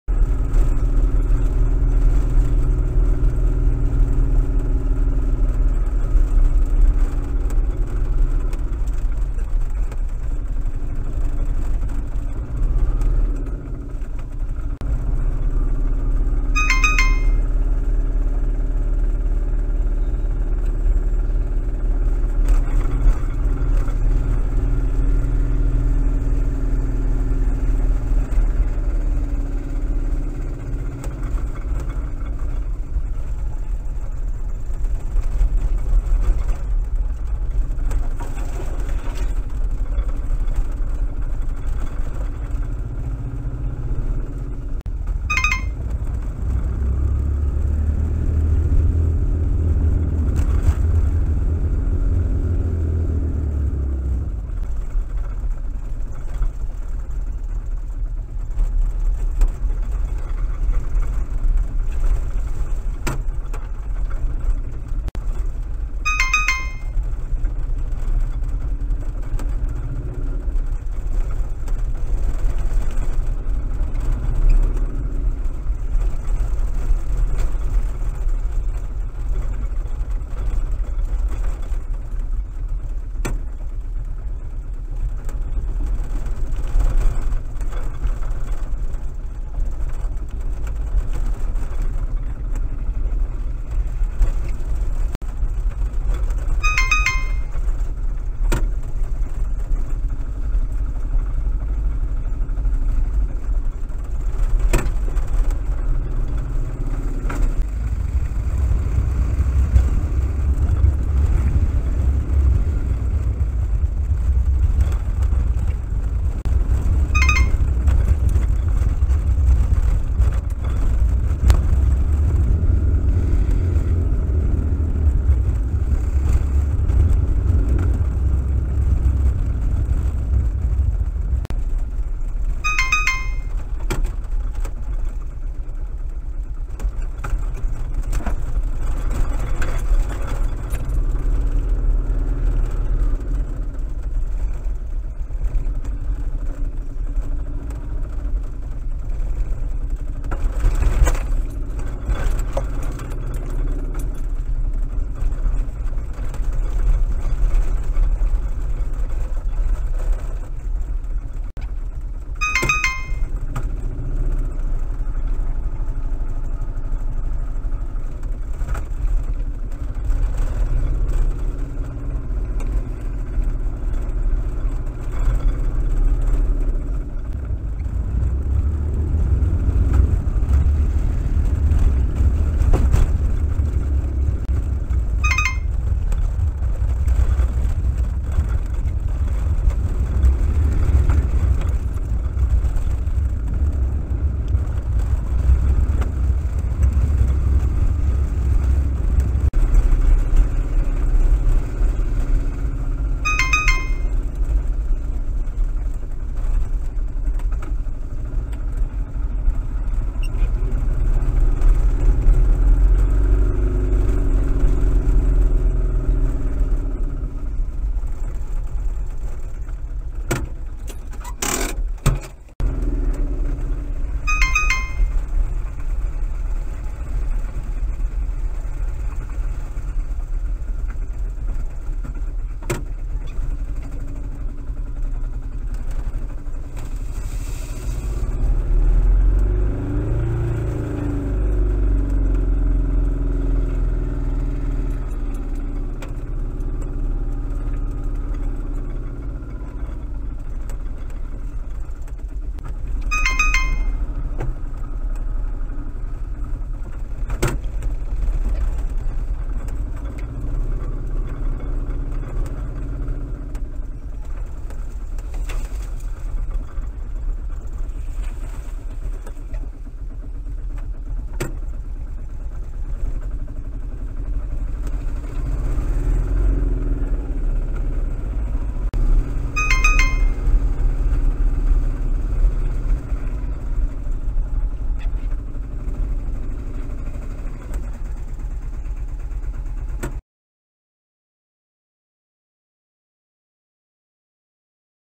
Turning Circles
Filmed entirely on E. & T.V.R. premises using the fitted "insurance" camera, the manoeuvrability of the railway's utilicon is shown off in what are actually some quite small spaces.
The turning circle of BEE 2309, the three-quarter ton diesel in service since 2007, is two feet greater than BEE 2247, the half-ton petrol-engined vehicle of 1982. The larger cab of the later type slightly reduces sideways movement of the front wheel.